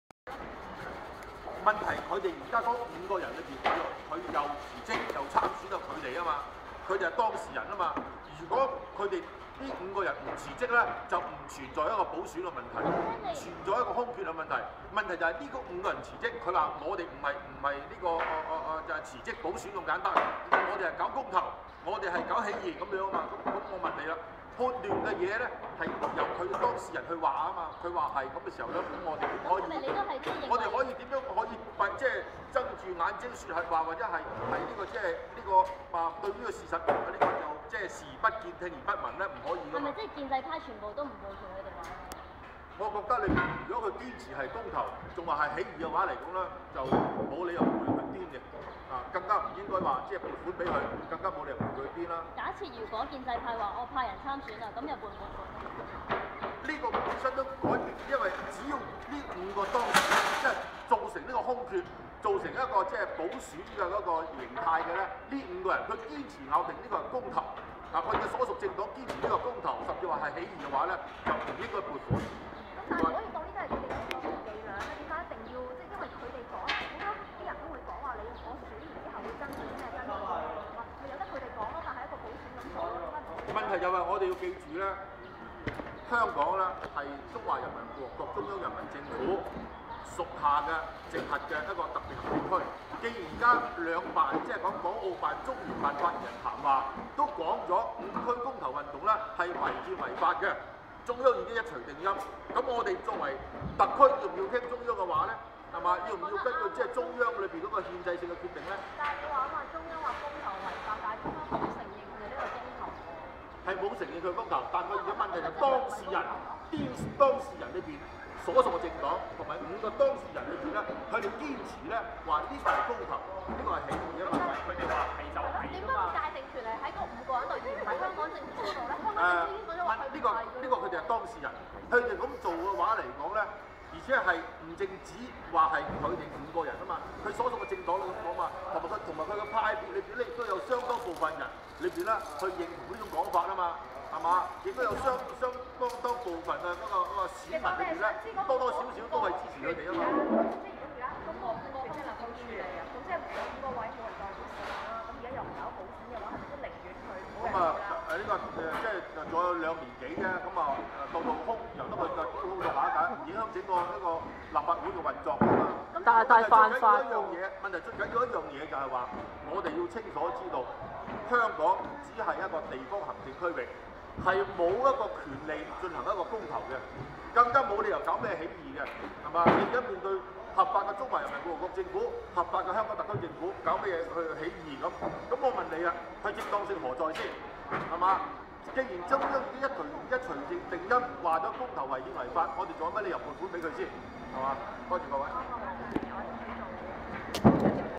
2010.01.24 - 《香港電台 城市論壇》(完結後) - 3 (劉夢熊 談五區公投)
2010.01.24  - CPPCC Member Mon-hung LEW comments on 'universial sufferage referendum' - 3

Venue: Bandstand, Victoria Park, Hong Kong
地點: 香港維多利亞公園涼亭

Mon-hung LEW 劉夢熊
CPPCC Member 中國全國政協委員